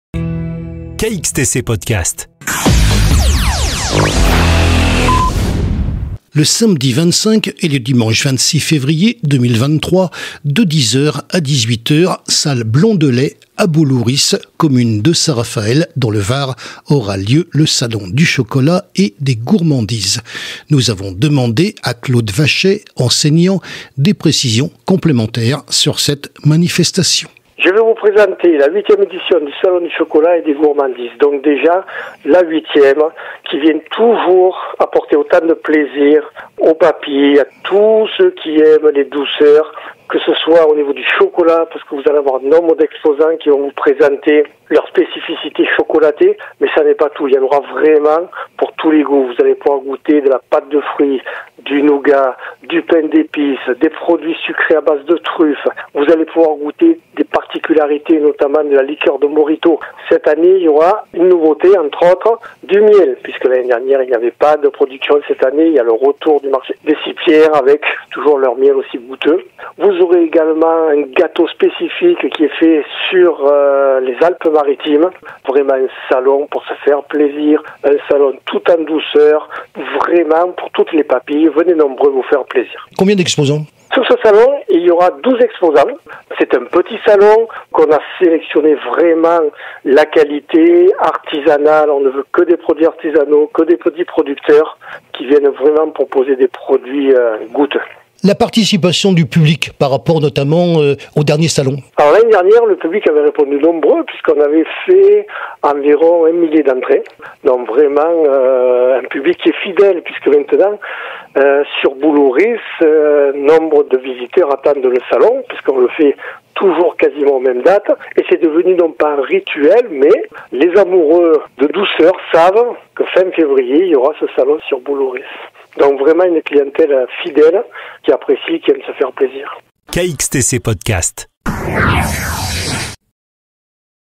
Capt Claude Vachet Salon du Chocolat et des Gourmandises Saint-Raphael Février 2023
Source:
Ville de Saint-Raphael
Salon du Chocolat et des Gourmandises Saint-Raphael Février 2023 - Captation Claude Vachet - Podcast Radiophonique